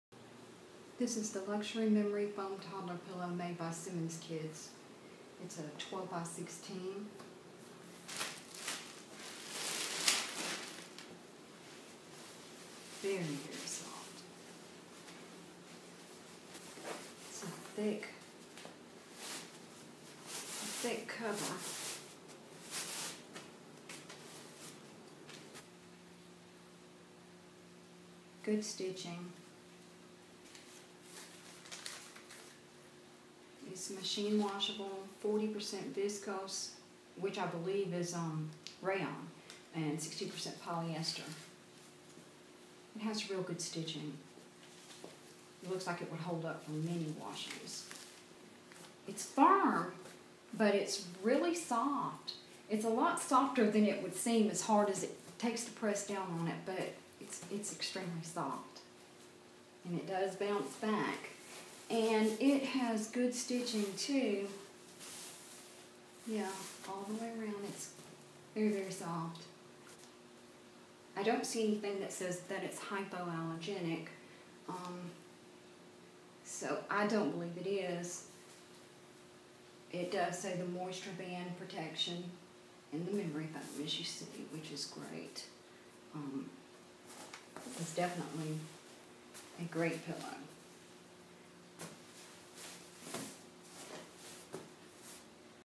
Simmons Kids Beautyrest Studio Luxury Memory Foam Toddler Pillow
Firm and Soft all in one pillow. No crackling cover just a VERY soft machine washable cover. Water and stain resistant. Excellent pillow!